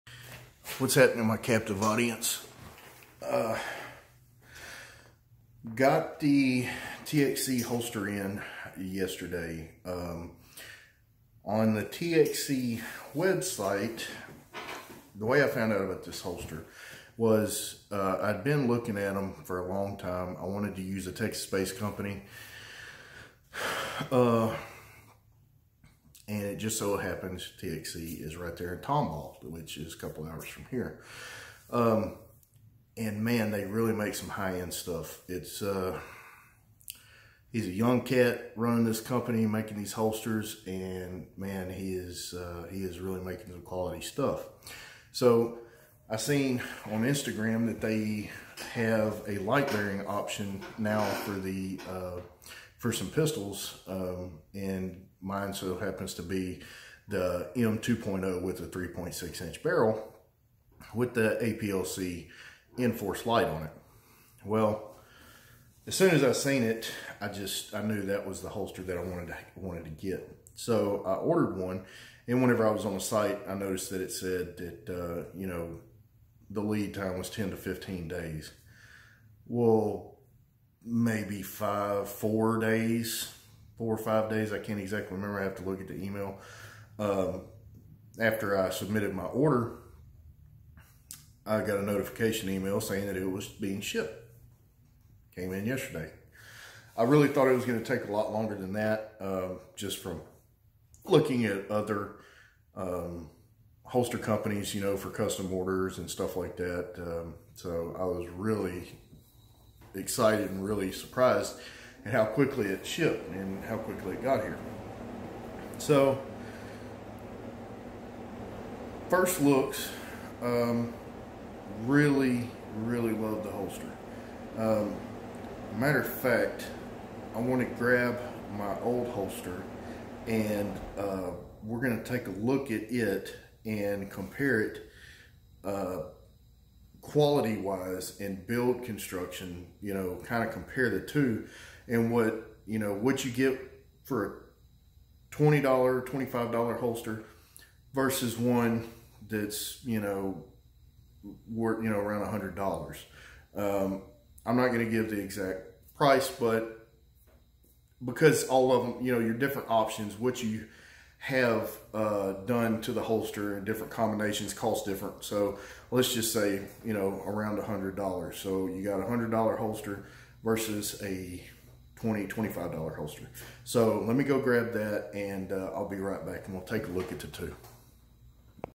Intro to TXC holster review
This is a short intro clip into the quick and impromptu review / comparison between the high end Txc holster and a cheapo 20 dollar holster .